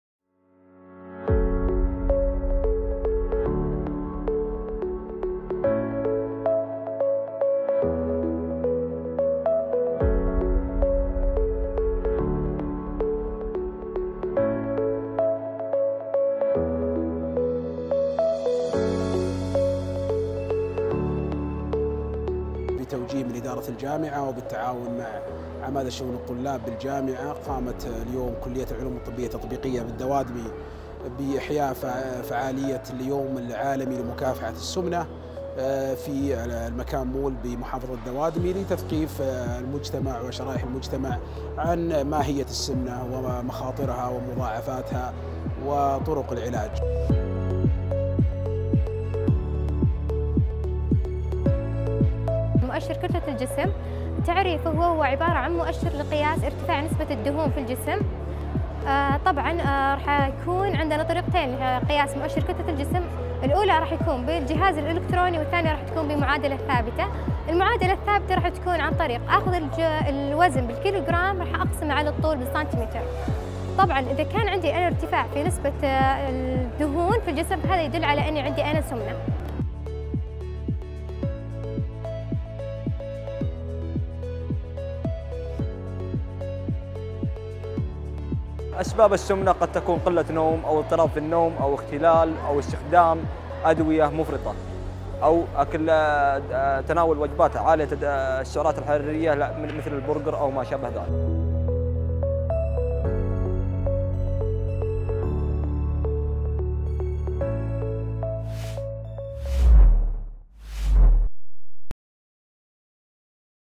أقامت كلية العلوم الطبية التطبيقية بـ الدوادمي حملة توعوية بمناسبة اليوم العالمي للسمنة...
أقامت كلية العلوم الطبية التطبيقية بـ الدوادمي بالتعاون مع عمادة شؤون الطلاب حملة توعوية بمناسبة اليوم_العالمي_للسمنة شملت فحوصات وتوعية بأضرار السمنة على صحة الفرد والمجتمع.

إنتاج الإدارة العامة للاتصال والإعلام بجامعة شقراء
Produced by the General Administration of Communication and Media at Shaqra  University